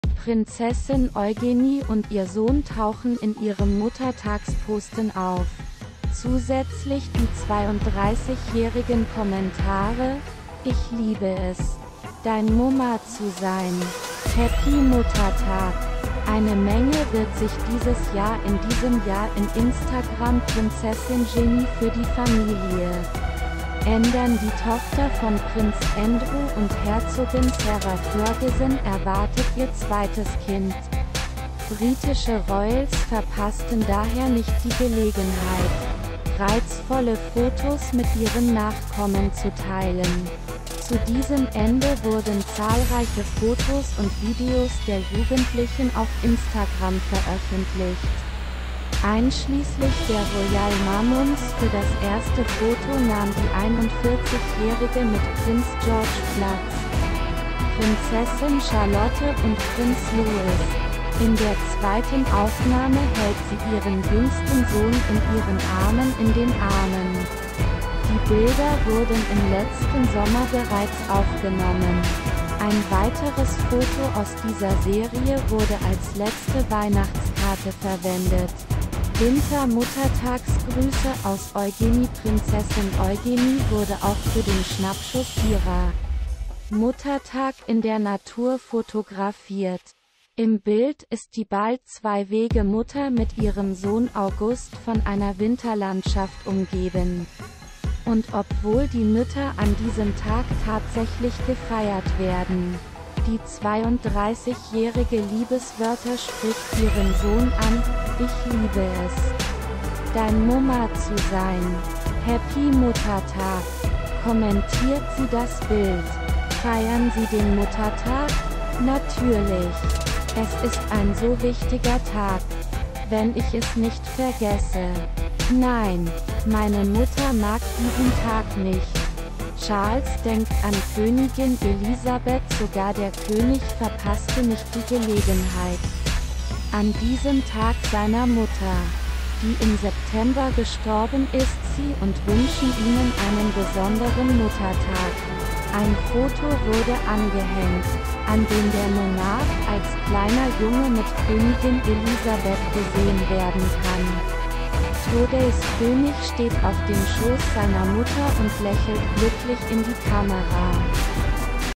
Muttertag: Prinzessin Eugenie teilt neues Bild mit Sohn August
Prinzessin Eugenie und ihr Sohn tauchen in ihrem Muttertagsposten auf. Zusätzlich die 32-jährigen Kommentare «Ich liebe es, dein Mumma zu sein! Happy Muttertag». Eine Menge wird sich dieses Jahr in diesem Jahr in InstagramPrinzessingenie für die Familie ändernDie Tochter von Prinz Andrew und Herzogin Sarah Ferguson erwartet ihr zweites Kind. Britische Royals verpassten daher nicht die Gelegenheit, reizvolle Fotos mit ihren Nachkommen zu teilen. Zu diesem Ende wurden zahlreiche Fotos und Videos der Jugendlichen auf Instagram veröffentlicht - einschließlich der Royal Mamums. Für das erste Foto nahm die 41-Jährige mit Prince George Platz, Prinzessin Charlotte und Prinz Louis . In der zweiten Aufnahme hält sie ihren jüngsten Sohn in ihren Armen in den Armen. Die Bilder wurden im letzten Sommer bereits aufgenommen. Ein weiteres Foto aus dieser Serie wurde als letzte Weihnachtskarte verwendet. Winter Muttertagsgrüße aus Eugenie Prinzessin Eugenie wurde auch für den Schnappschuss ihrer Muttertag in der Natur fotografiert. Im Bild ist die bald zwei Wege -Mutter mit ihrem Sohn August von einer Winterlandschaft umgeben. Und obwohl die Mütter an diesem Tag tatsächlich gefeiert werden, die 32-Jährige Liebeswörter spricht ihren Sohn an «Ich liebe es, dein Mumma zu sein! Happy Muttertag», kommentiert sie das Bild. Feiern Sie den Muttertag? Natürlich! Es ist ein so wichtiger Tag. Wenn ich es nicht vergesse. Nein, meine Mutter mag diesen Tag nicht. Charles denkt an Königin Elizabeth Sogar der König verpasste nicht die Gelegenheit, an diesem Tag seiner Mutter, die im September gestorben istSie und wünschen Ihnen einen besonderen Muttertag. Ein Foto wurde angehängt, an den der Monarch als kleiner Junge mit Königin Elizabeth gesehen werden kann. Todays König steht auf dem Schoß seiner Mutter und lächelt glücklich in die Kamera.